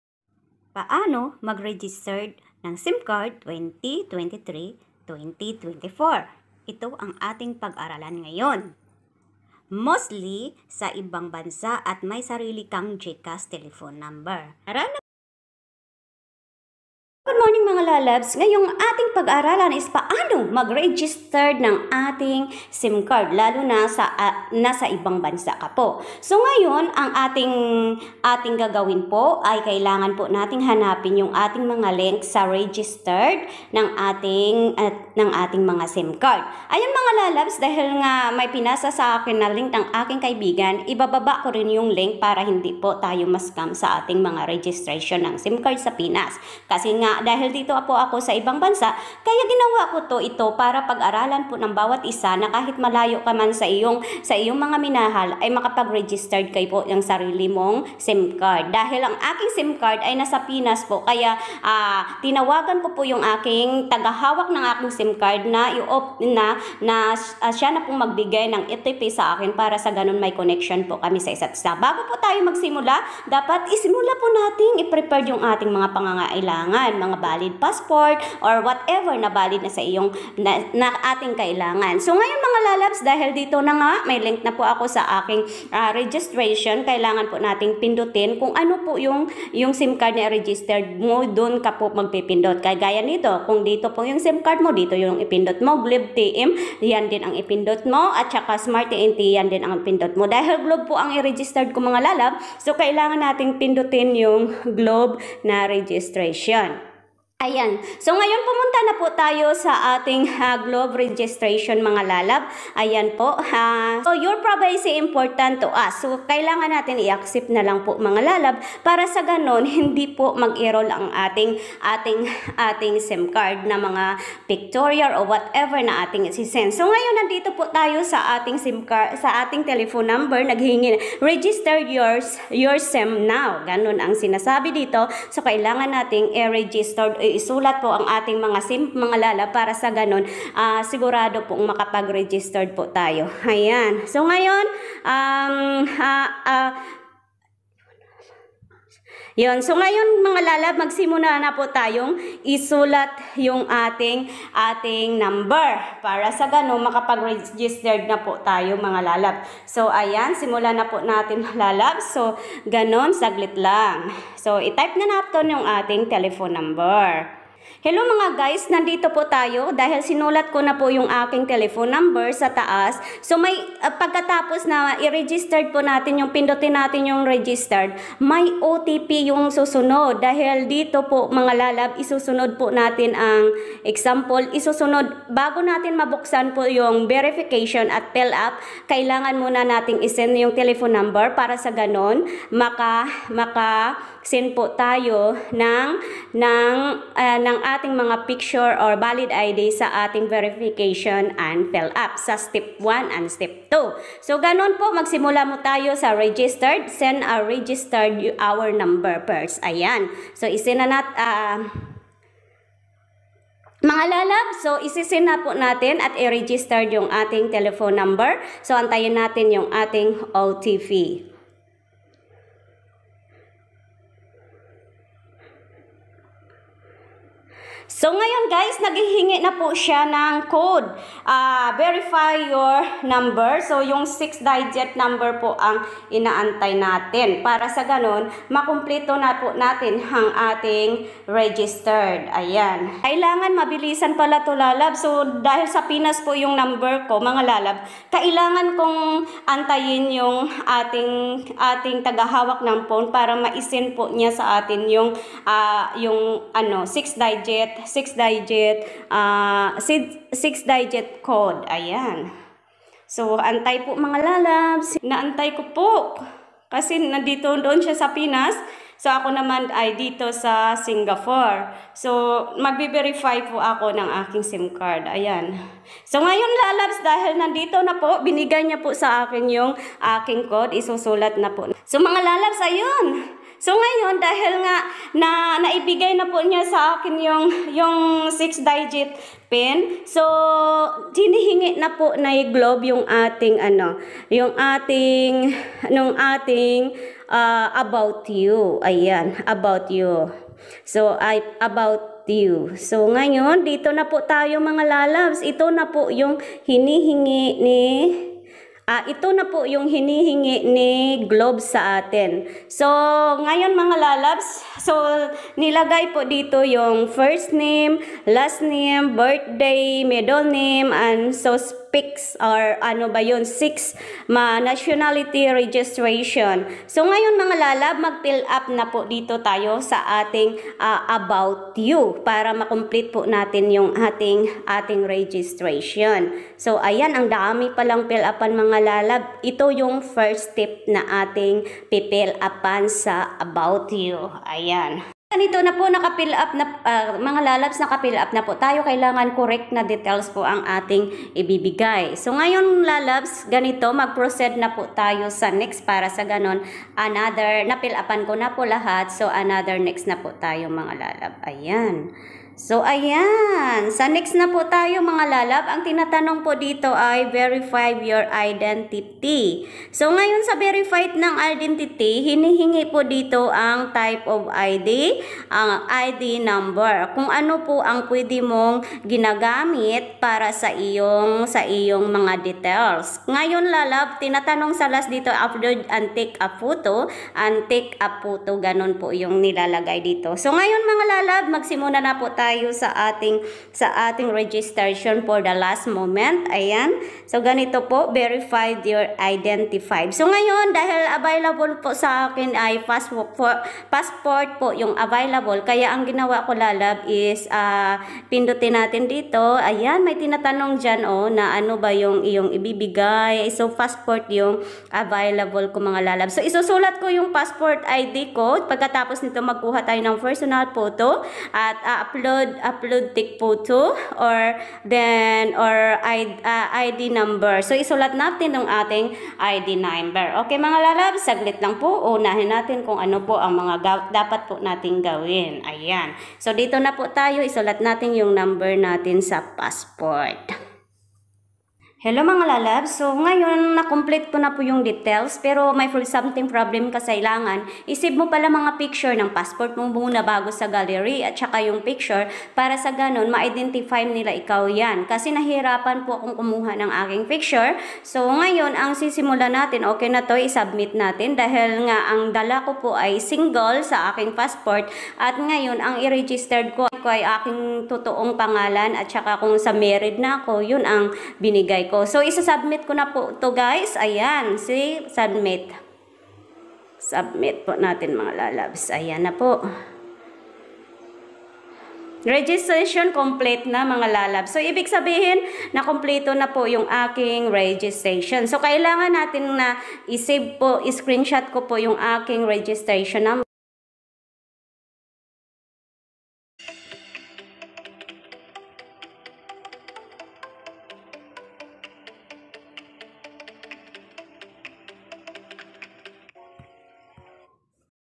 PAANO MAG REGISTER NG SIM CARD 2023-2024 STEP BY STEPTUTORIAL
This tutorial is about how to Registered Philippines sim card so that we identify  who's the owner for our safety identification Tel #  mostly sa gumagamit ng Gcash para di ma deactivate Ang iyong # at di masayang Ang iyong pira pag aralan po natin ng step by step fallowing my tutorial salamat po .